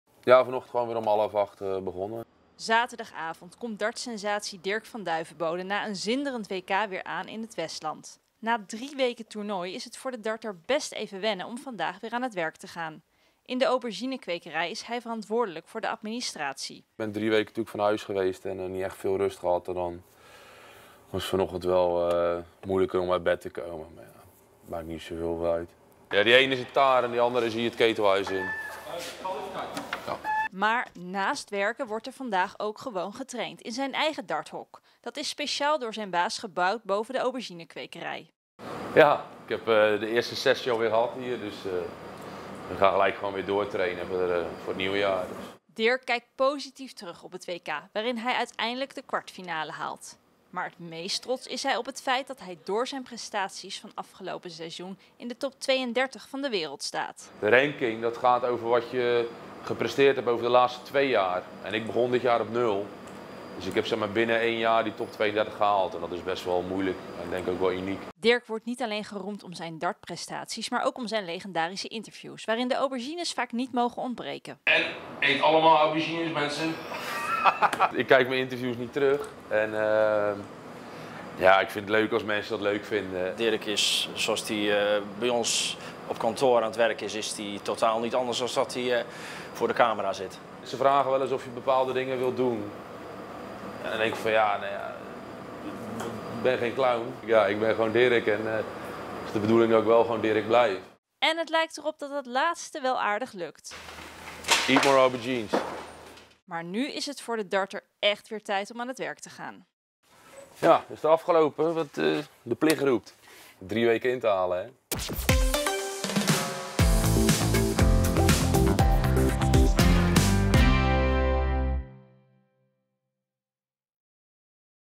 Dirk van Duijvenbode terug en weer aan het werk
Dirk van Duijvenbode werd op nieuwjaarsdag uitgeschakeld op het WK darts in Londen. Hij verloor in de kwartfinale van de Schot Gary Anderson. Het was een partij waarin de 28-jarige 's-Gravenzander zijn draai niet kon vinden, al begon hij goed. Lang treuren doet hij niet. Dirk ging maandag gewoon weer aan het werk in de auberginekwekerij in 's-Gravenzande.